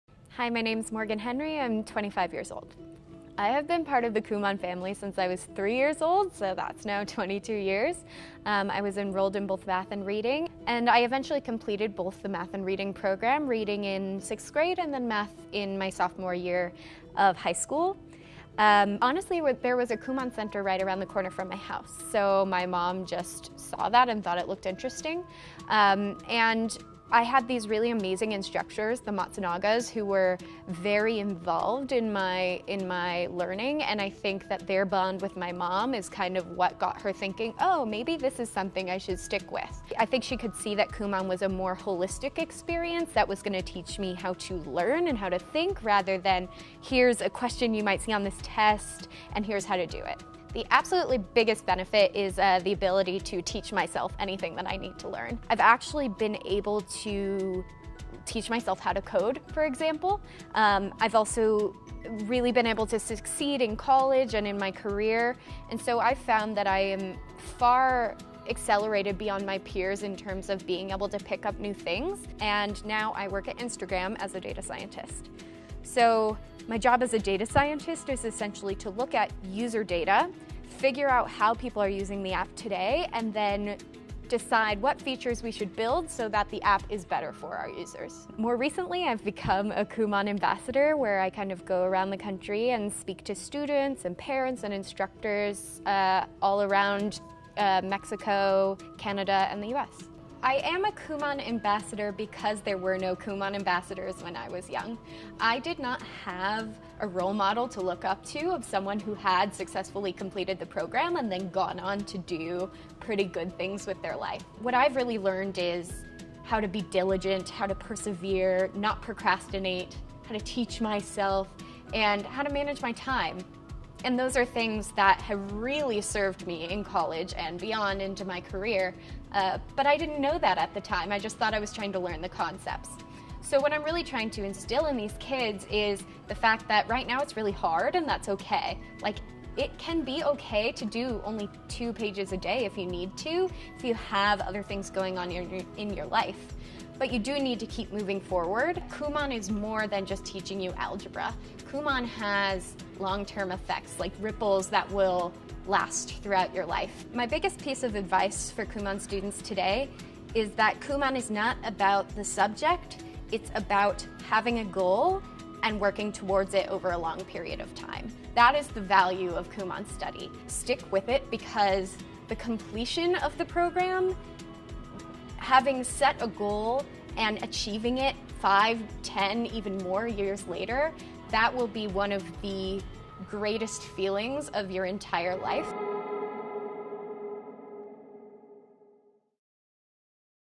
Kumon Ambassador Discusses Benefits of Kumon Method
Morgan Henry has been part of the Kumon Family for over 20 years! She first enrolled in the program at the age of three and is now a dual program completer and Kumon Ambassador. Aside from her daily job as a Data Scientist at Instagram, she also travels around the country to talk about her experience with the Kumon Method.